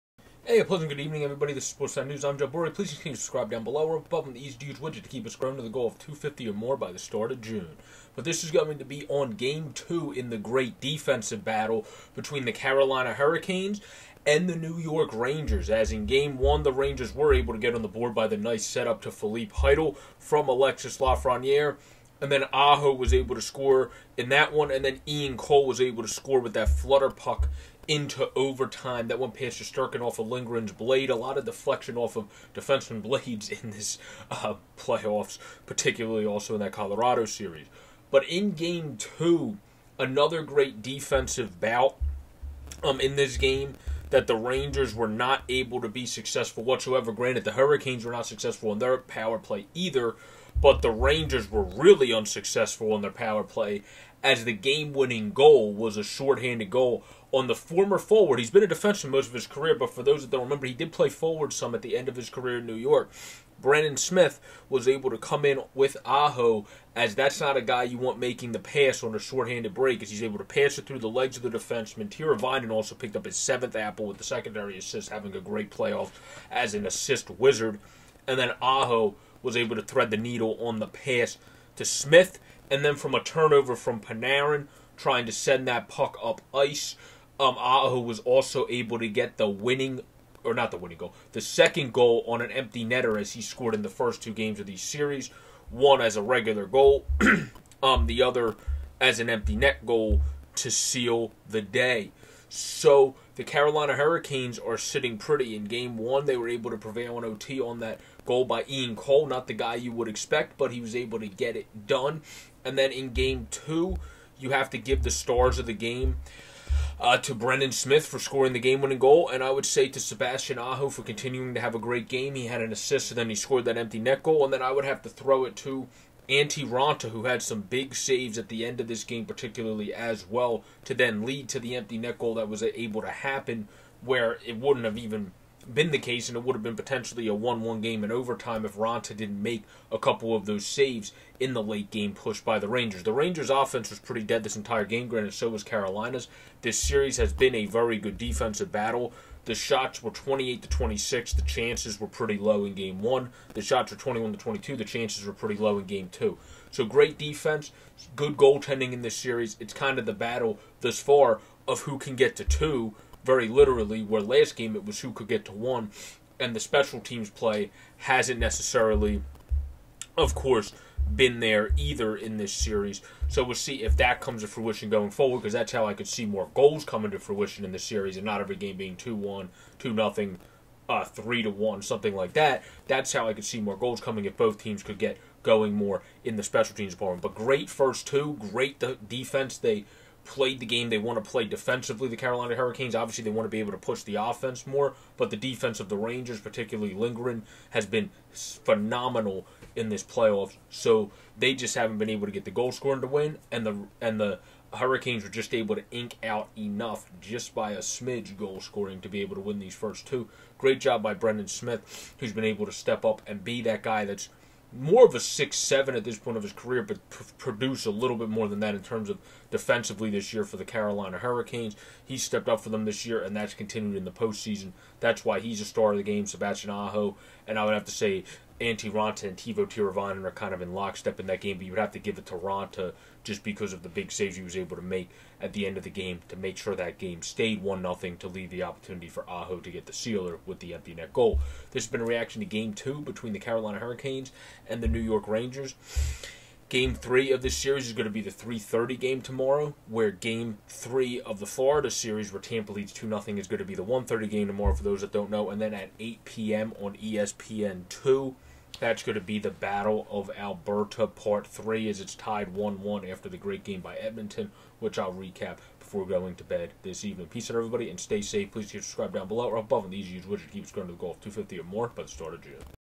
Hurricanes Take Another Defensive Battle in Game 2 Thanks to Smith's Shortie
Pro Joe is back to recap part 2 of another great defensive battle in the Rangers and Canes series in this Hurricanes Take Another Defensive Battle in Game 2 Thanks to Smith's Shortie. He breaks down the defense of both teams, struggling powerplays, plus more than led to the Canes taking a 2-0 lead going into tomorrow in MSG in this series. Thanks and enjoy the hockey